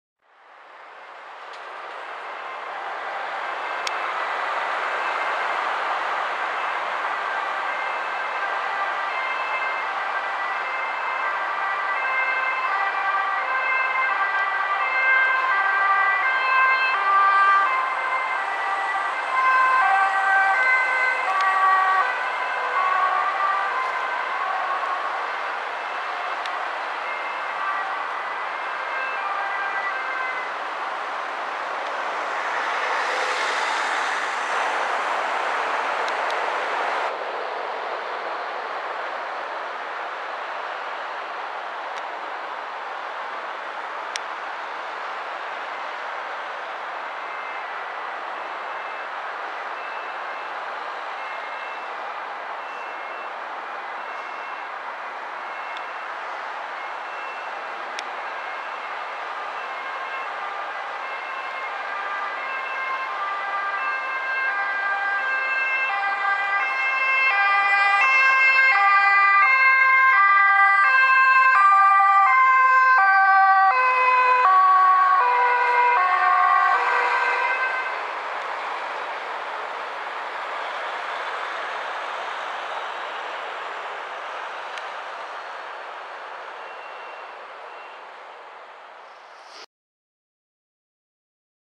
2X A1: Ambulance 20-144 komt met spoed ter plaatse bij een ongeval op de A58 (HD)
A1: Ambulance 20-144 komt met spoed ter plaatse bij een ongeval op de A58.
Hij rijd eerst lang het ongeval en komt daarna ter plaatse nadat hij heeft gedraaid op het viaduct.